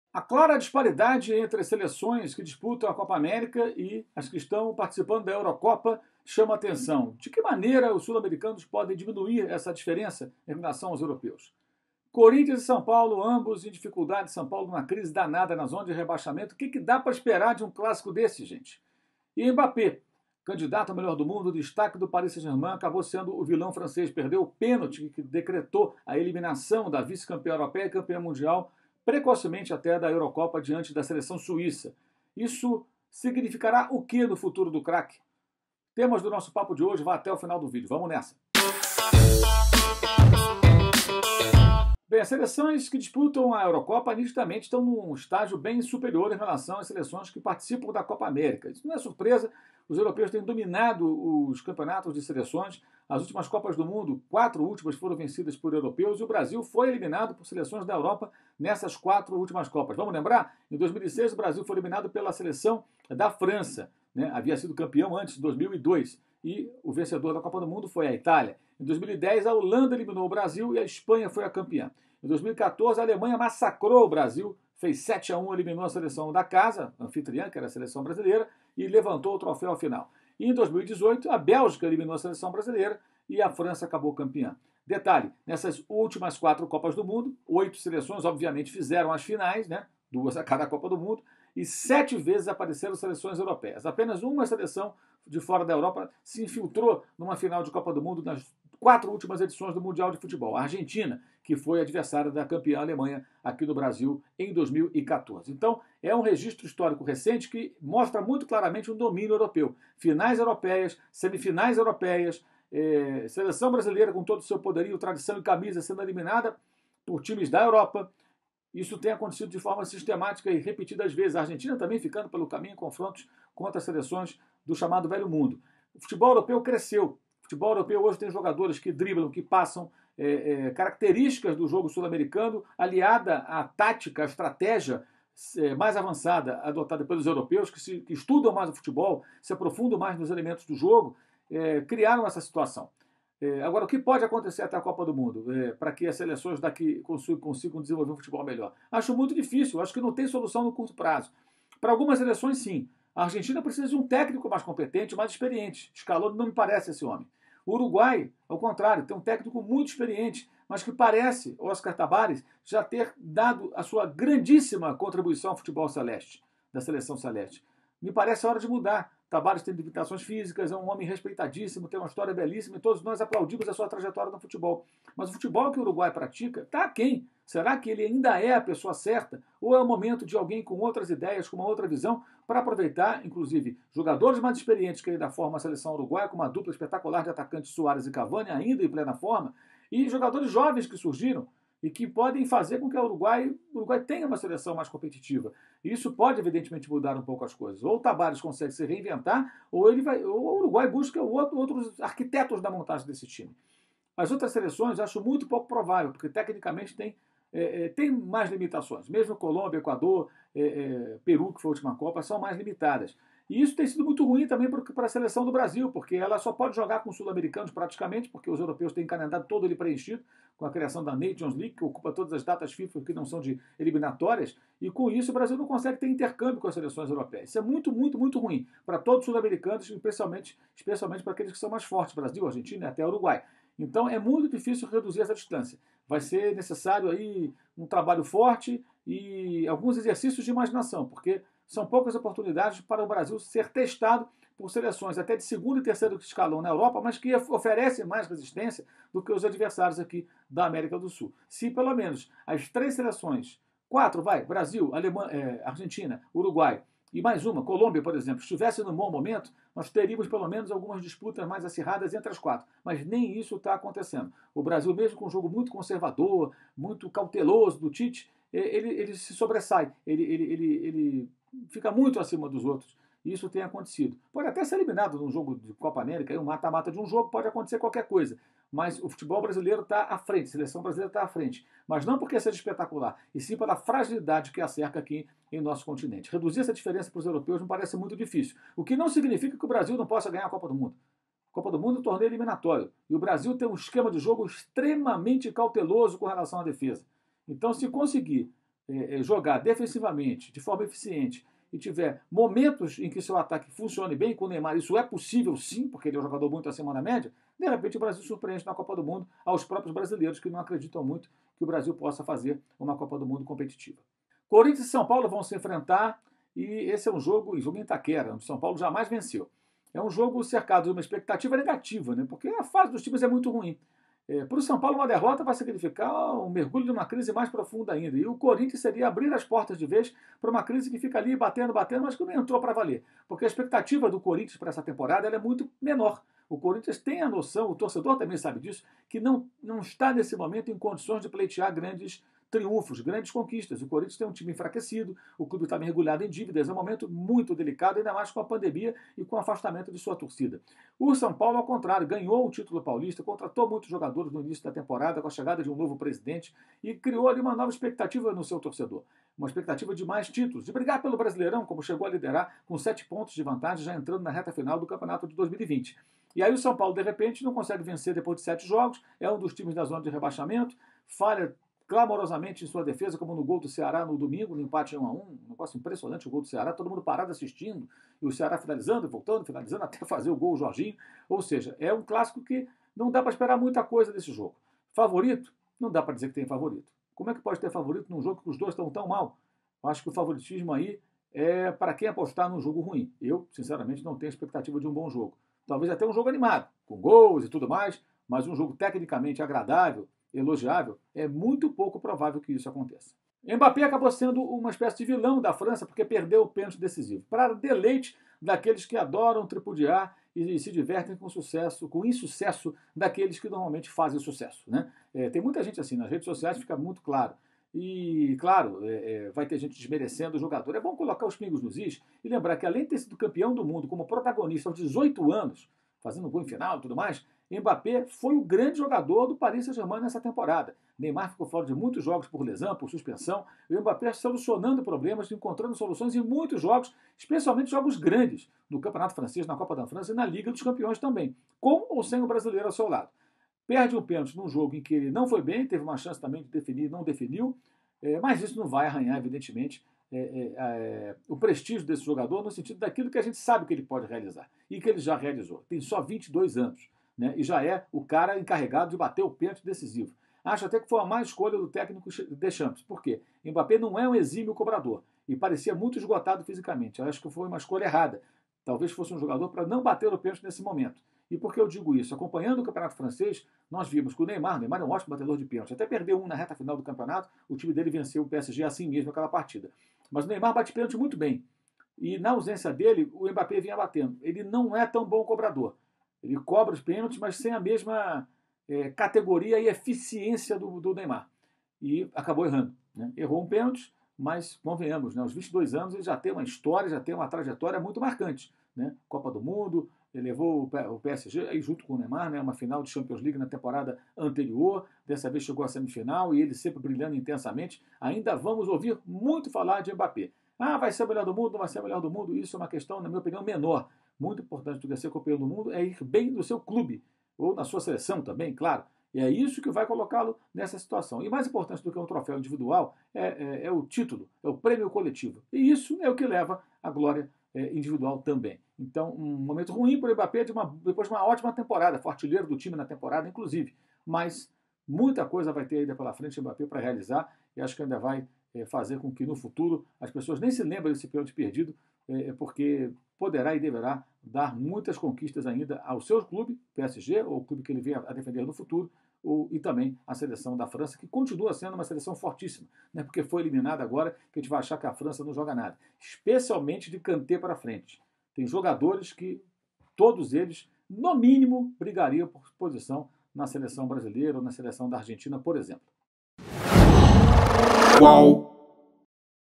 Mauro: Corinthians e São Paulo é um jogo cercado de expectativa negativa
Em tempos de Eurocopa e Copa América, o jornalista Mauro Cezar Pereira, colunista do UOL Esporte,  analisa a diferença técnica que separa atualmente o futebol das seleções europeias do das sul-americanas, fala sobre a expectativa negativa que cerca o clássico entre Corinthians e São Paulo pelo Campeonato Brasileiro e explica porque perder o pênalti na eliminação da França na Euro não arranha o prestígio de Mbappé.